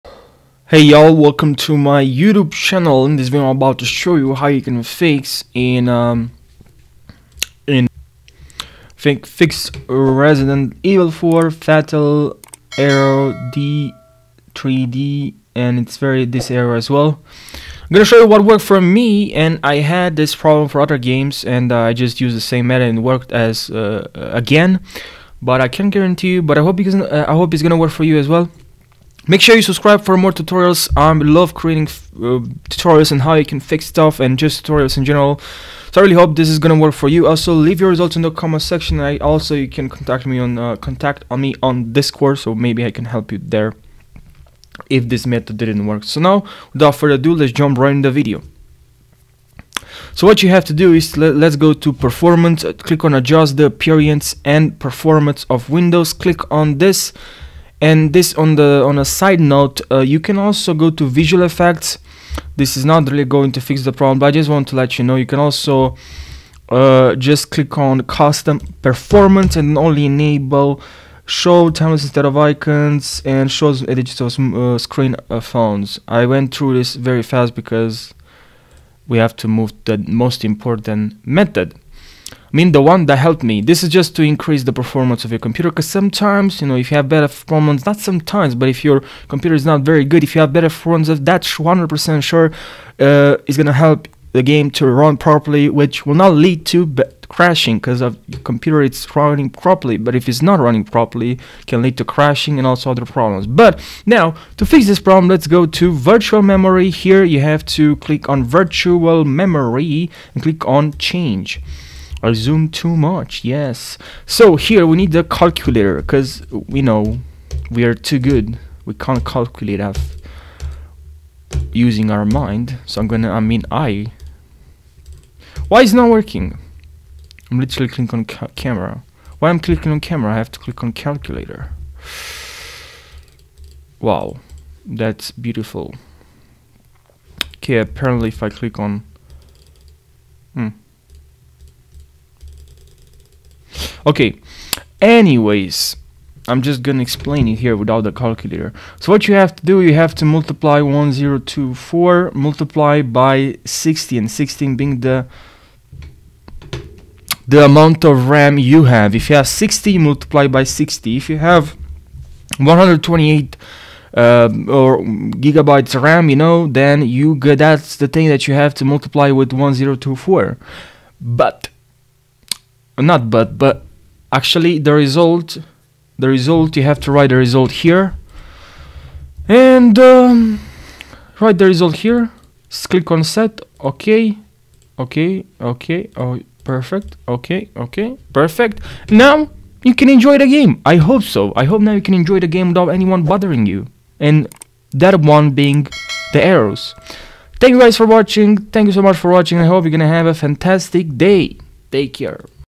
How to Fix Resident Evil 4 Fatal D3D Error Fix RE4 Fatal D3D Error
In this video, we'll show you how to fix the fatal D3D error in Resident Evil 4. If you're experiencing this error while trying to launch the game, don't worry, we've got you covered!

First, we'll walk you through the possible causes of the error and then provide a step-by-step guide to fix it. We'll show you how to update your graphics card driver, install DirectX, and change the game's compatibility mode.

With our easy-to-follow instructions, you'll be able to quickly resolve the fatal D3D error and get back to playing your favorite game without any further interruptions.

So, whether you're a seasoned RE4 player or just getting started, be sure to watch this video and learn how to fix the fatal D3D error in Resident Evil 4.